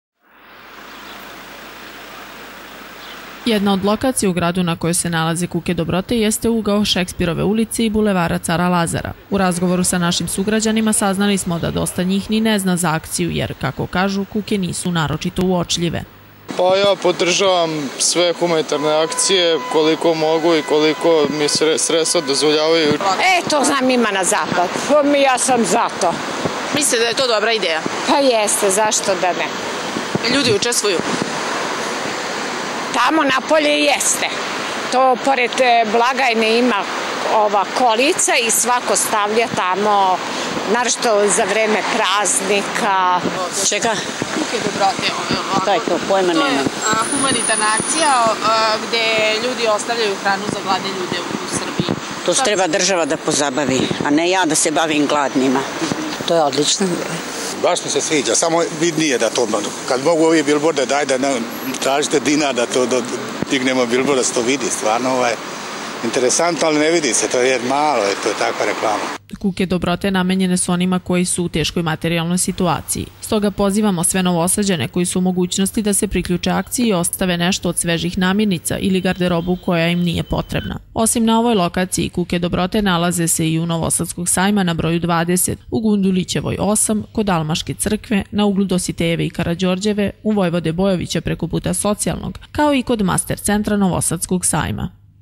TV KANAL 9, NOVI SAD: "KUKE DOBROTE" I U NOVOM SADU
Humanitarna akcija "Kuke dobrote" proširila se na celu Srbiju. Desetak ovih kuka okačeno je i na različitim lokacijama i u našem gradu. Ekipa Kanala 9 proverila je koliko su Novosađani upoznati sa ovom akcijom i da li ostavljaju namirnice na ovim kukama.